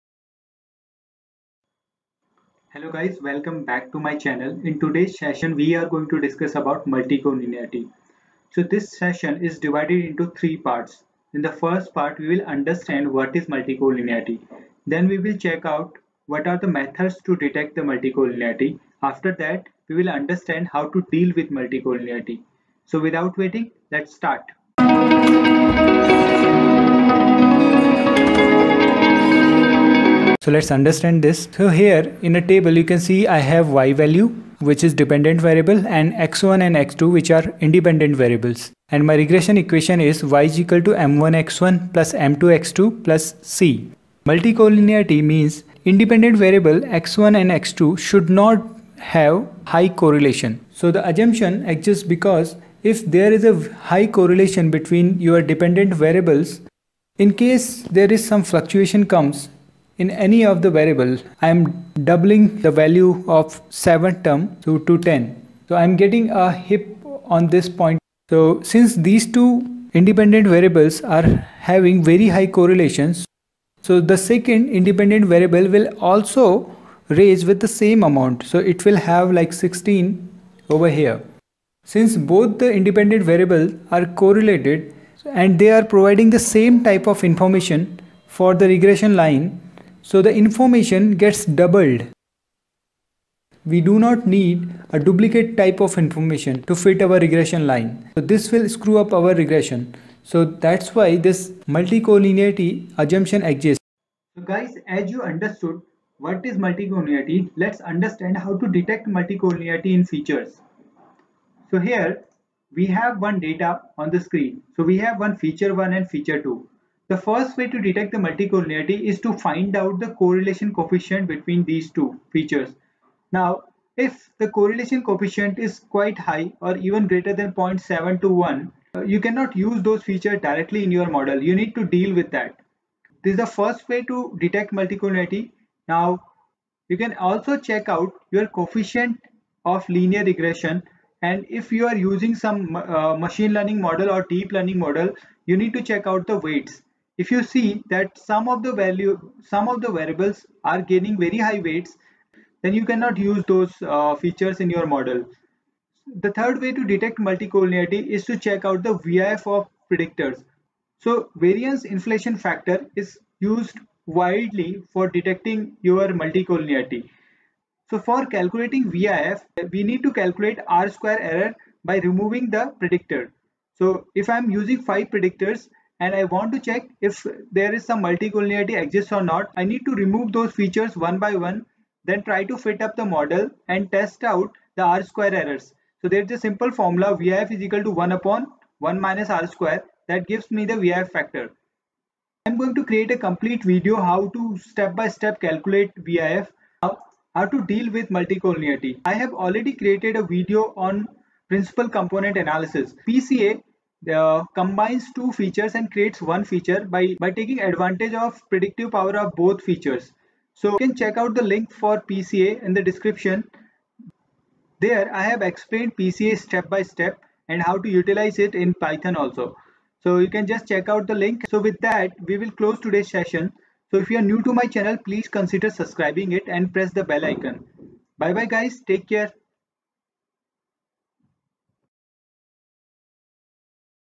Multicollinearity | Multicollinearity in Linear Regression
In this video I have explained details about multicollinearity which is a very important cause of failure of models in data science. Here I have not only explained about multicollinearity  but also provided the ways to detect and handle it.

Here is the link for Principle component analysis(PCA)

This video is helpful if you are looking for
1. Linear regression
2. basics of data science